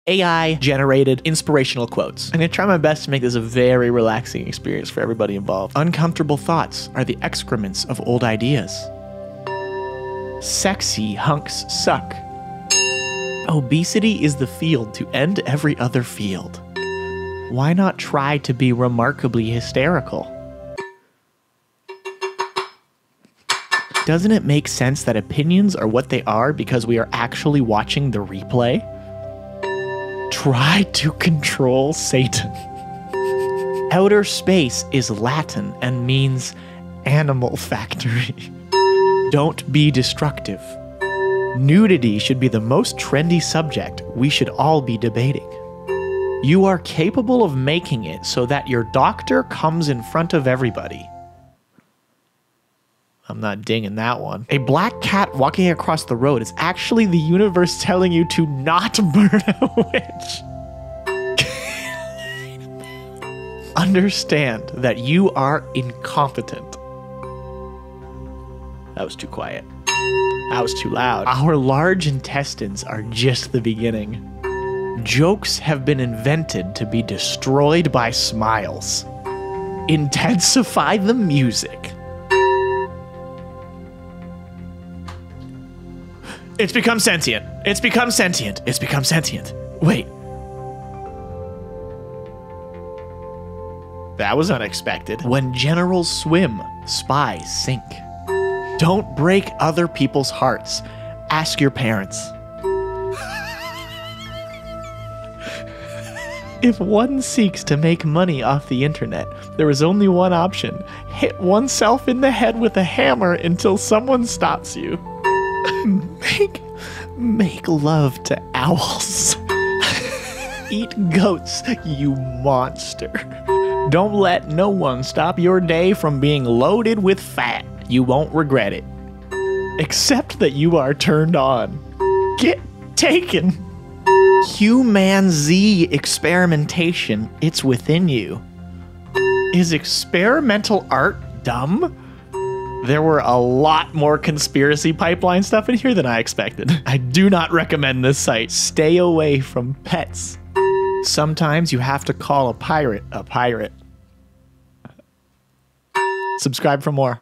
Reacting to Randomly Generated Inspirational Quotes [EXTENDED VERSION]
Relax as we react to random generated inspirational quotes from Inspirobot. Be inspired. Be motivated. Be calm.

In this video I use therandom inspirational quote generator called Inspirobot. It generates random inspirational quotes!